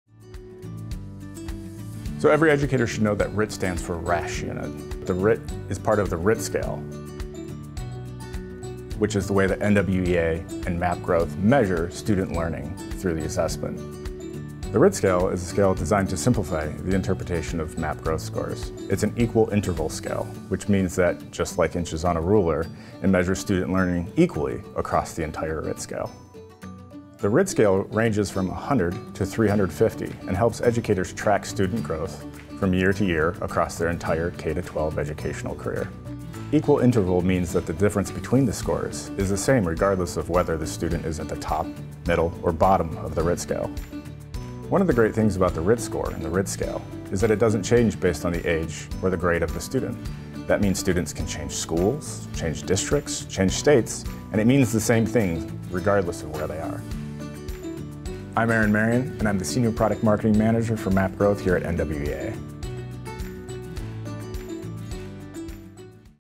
Understanding your MAP Growth RIT scores in 60 seconds
"One of the great things about the RIT score and the RIT scale is that it doesn't change based on the age or the grade of the student. That means students can change schools, change districts, change states, and it means the same things regardless of where they are."

Aaron Marion, Senior Product Marketing Manager for MAP Growth at NWEA, discusses the RIT scale and how it helps measure student learning.